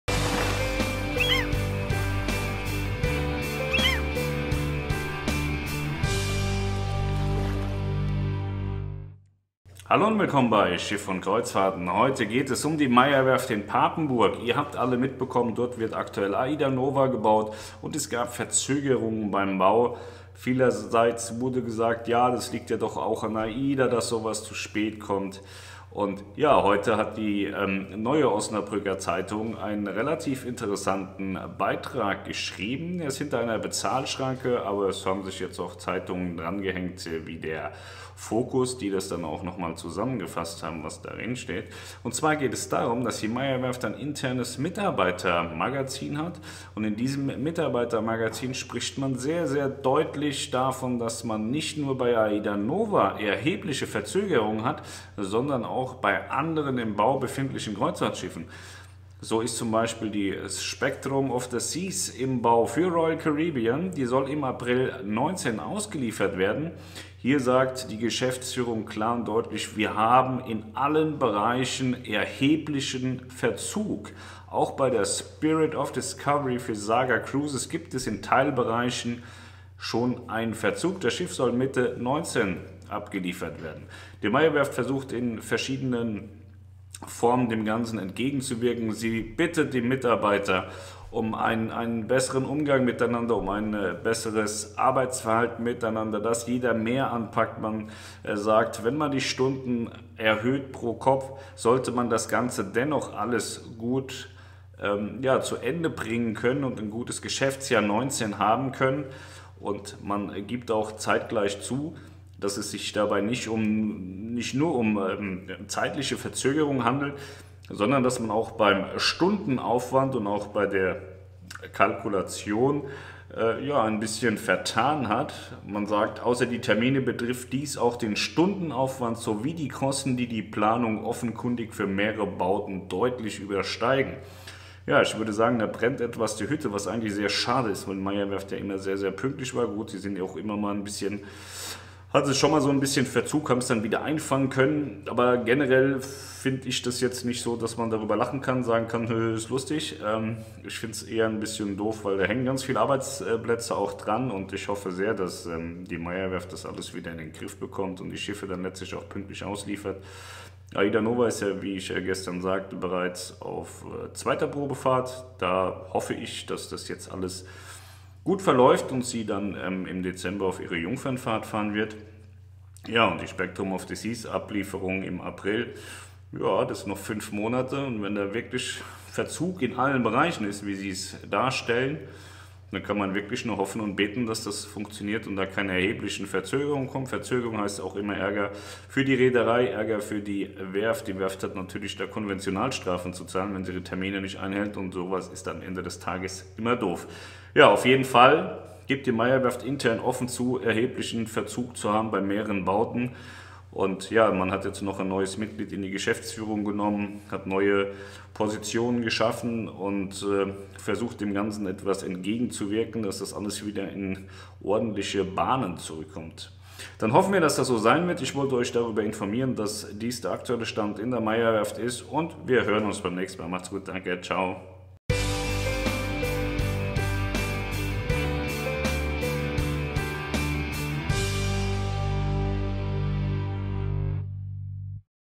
[Meyer Werft in Verzug] AIDAnova | Spectrum of the Seas | Spirit of Discovery
Moin, ihr findet uns ab sofort und ausschließlich Eine weitere Hiobsbotschaft aus der Meyer Werft erreichte heute die Allgemeinheit. Die „neue Osnabrücker Zeitung“ teilte heute mit, was die Meyer Werft Geschäftsführung in ihrem jüngsten internen Mitarbeiterheft veröffentlicht hat.

Hier steht geschrieben, dass die Meyer Werft nicht nur bei der AIDAnova zeitliche Probleme hat, sondern auch bei der Spectrum of the Seas „erheblicher Verzug in allen Bereichen“ vorliegt. Das Schiff für die US-Reederei Royal Caribbean soll im April 2019 abgeliefert werden.

Auch in einigen Bereichen der Spirit of Discovery für Saga Cruises sei man in Verzug. Das bedeutet, dass die Meyer Werft in Papenburg derzeit bei allen Neubauten in den Docks zeitliche Probleme hat. Darüber hinaus teilt man auch mit, dass nicht nur der Stundenaufwand sowie die Kosten sondern auch die Planungen für mehrere Neubauten deutlich den Forecast übersteigen.

Die Meyer Werft Geschäftsführug appeliert an die Mitarbeiter, dass wenn sich die Kollegen gegeneinander helfen und unterstützen, sowie eine Vereinbarung zur freiwilligen Mehrarbeit treffen, die besagt, dass Mitarbeiter in kritischen Bereichen und Zeiten auch 45 Stunden pro Woche arbeiten, dass das Ganze dann insgesamt doch wieder gerade gerückt werden könnte.

Weiterhin soll das Team aufgestockt werden der Meyer Werft und mehr junge Menschen ausgebildet werden um auch den wachsenden Anforderungen gerecht zu werden. Man will aus Fehlern lernen und diese nicht auf dem Rücken der Belegschaft austragen.